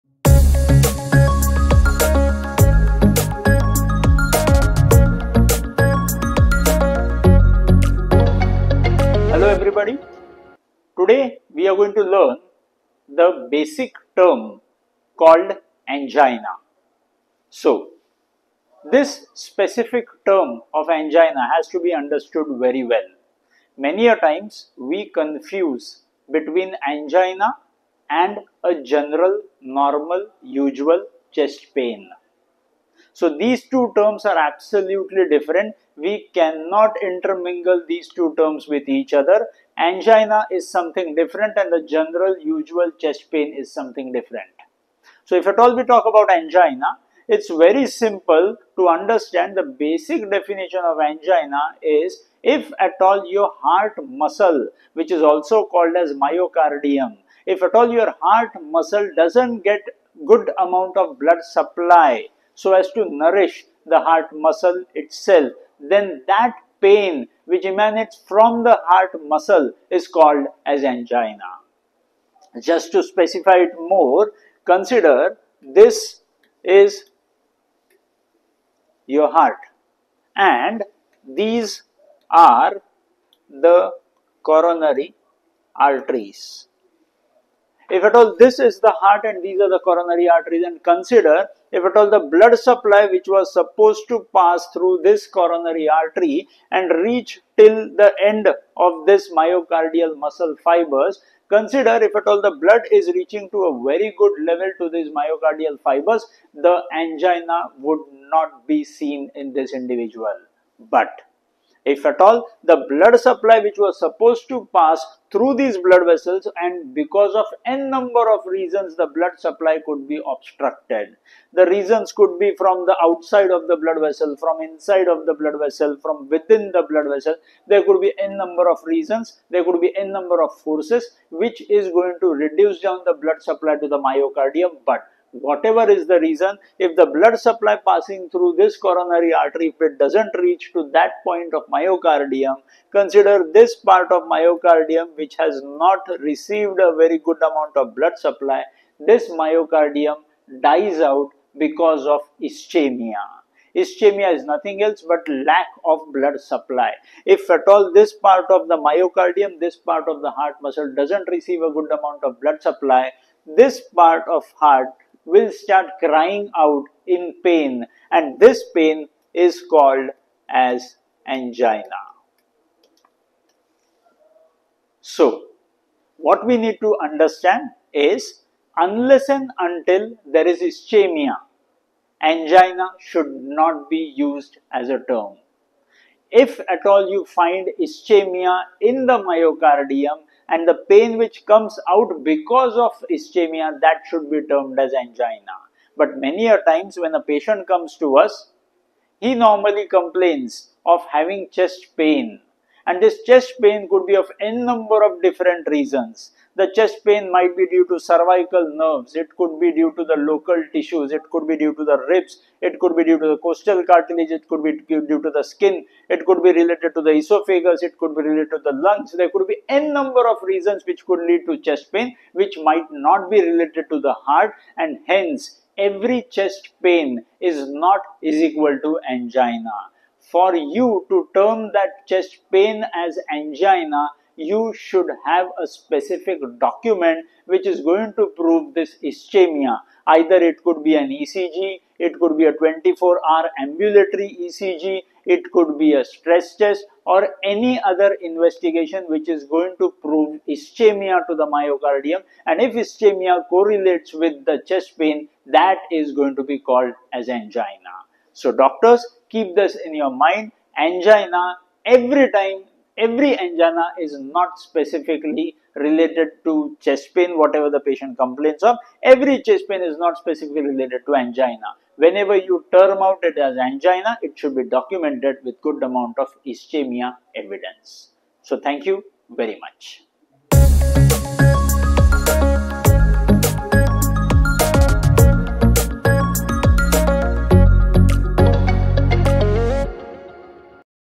Understanding Angina: Differentiating Chest Pain from Myocardial Ischemia | Series for Doctors
In this comprehensive video, Dr. Rohit Sane, a leading expert in preventive cardiology, explains the crucial difference between angina and general chest pain, a topic that every healthcare professional should be well-versed in. Angina, specifically linked to myocardial ischemia, occurs when the heart muscle, or myocardium, doesn't receive an adequate blood supply, leading to a unique type of pain that must be accurately identified and treated.

Dr. Sane takes you through the basic definition of angina, the physiological causes behind it, and how it differs significantly from other types of chest pain, which might be related to non-cardiac issues such as the cervical nerves, rib cage, esophagus, or even the lungs. He emphasizes the importance of using diagnostic tools like ECG, 24-hour ambulatory ECG, and stress tests to document ischemia before diagnosing angina.

This video is a must-watch for doctors, cardiologists, medical students, and healthcare providers who want to sharpen their diagnostic skills and improve patient outcomes. Ensure you're equipped with the knowledge to distinguish between true angina and other chest pain, thereby avoiding misdiagnosis and ensuring proper treatment.